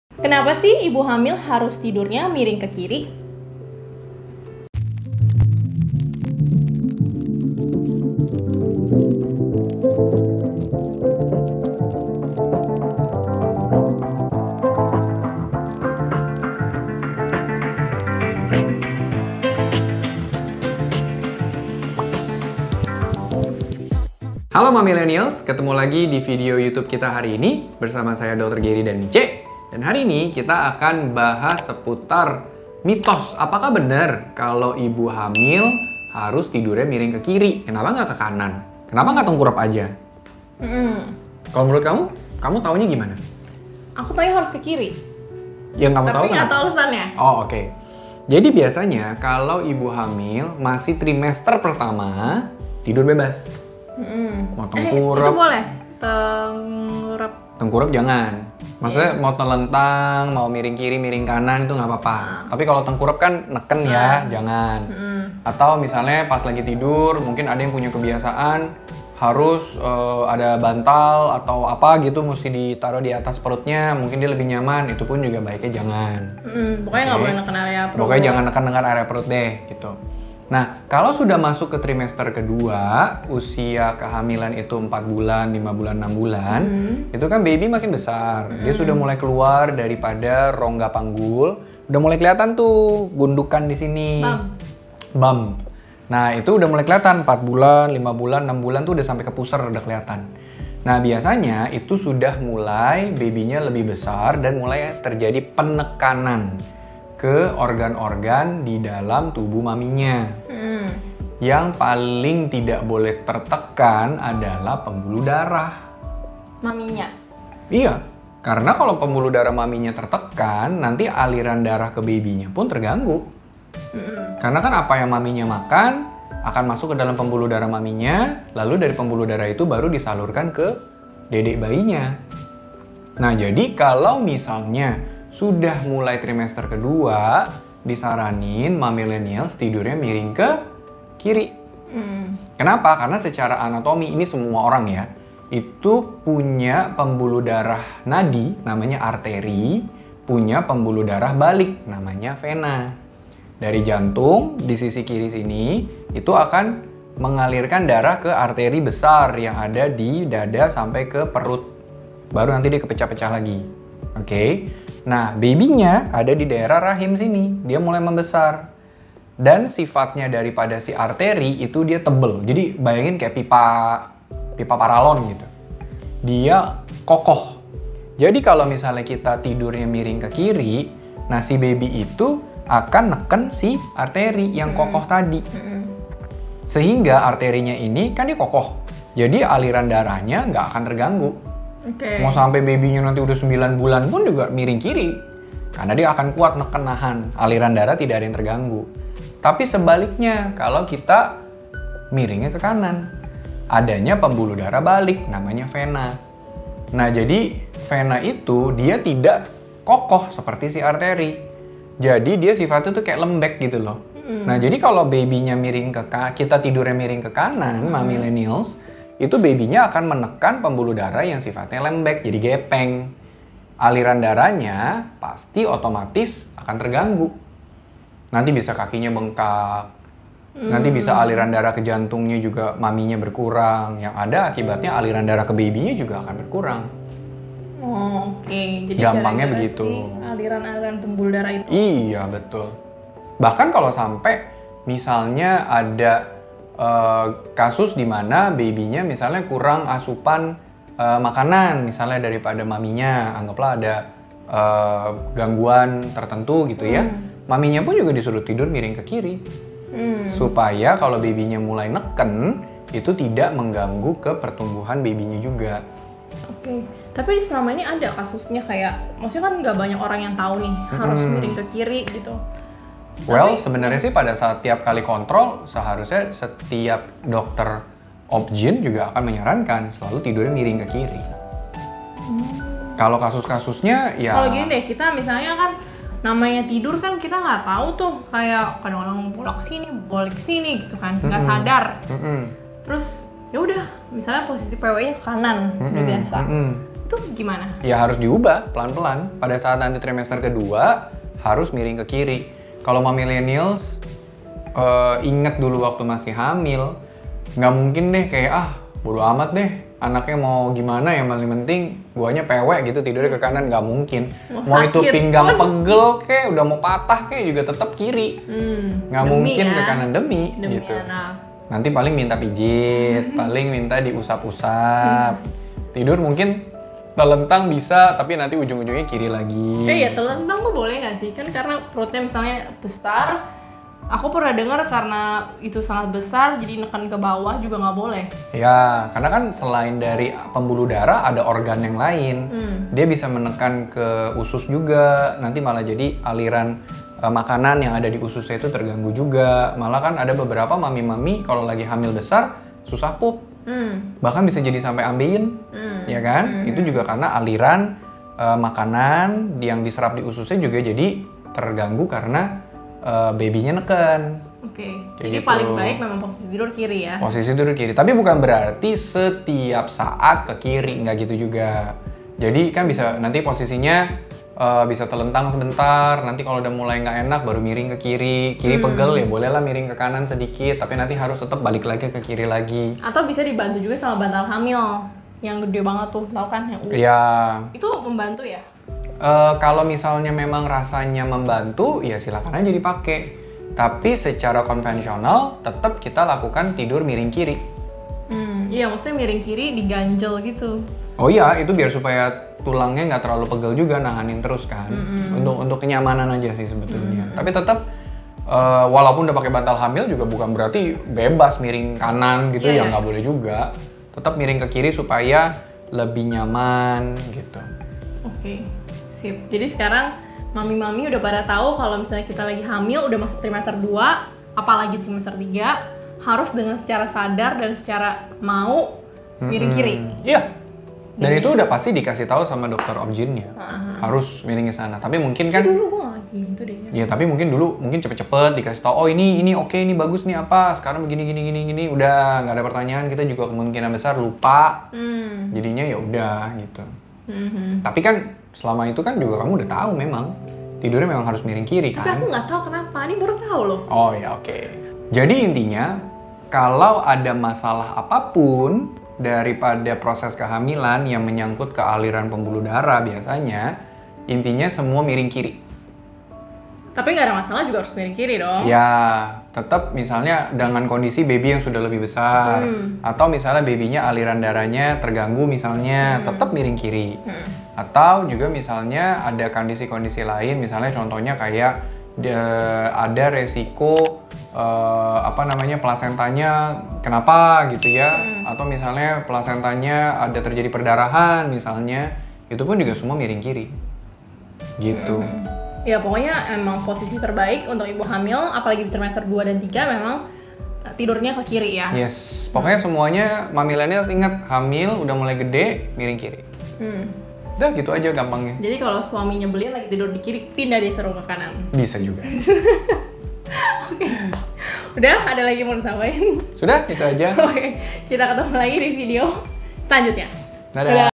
Ini Alasan Bumil Harus Tidur Miring Kiri!
Halo Mamilenials!

Uda pada tau belum kalau bumil wajib tidur MiringKiri? Apa sih alasannya? Ternyata ada alasan medis lho di balik anjuran itu. Nonton videonya sampai habis ya!